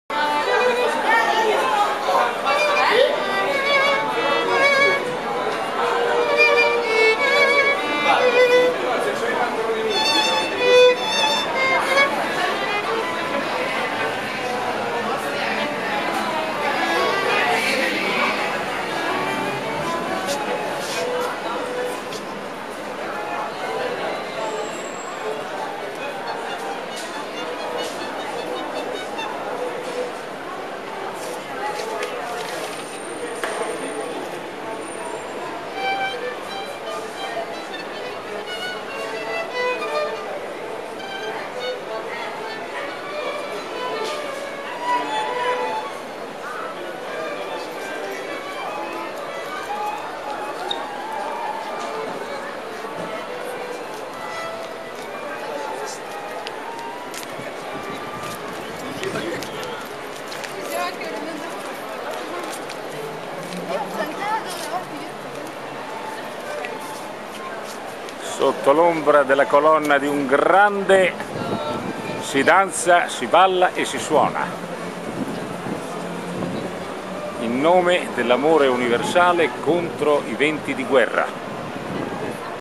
Flash Mob 2
Musicisti che improvvisano nel cuore di Roma.